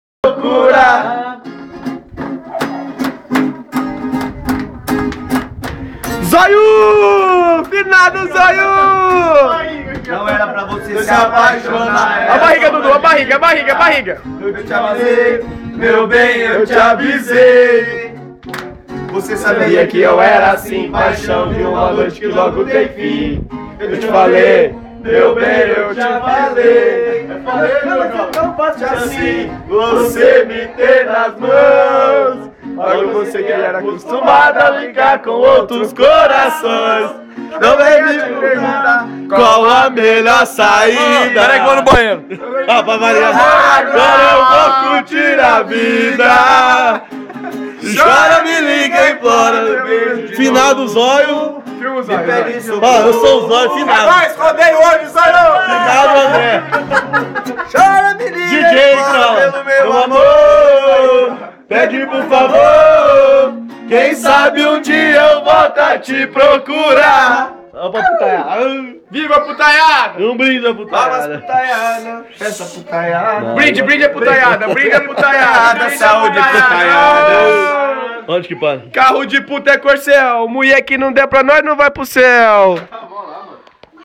Os Terriveis cantando sertanejo (parte2)
Pescaria que ja mais saira de nossa memoriaa. Pena que nao tem peixeee...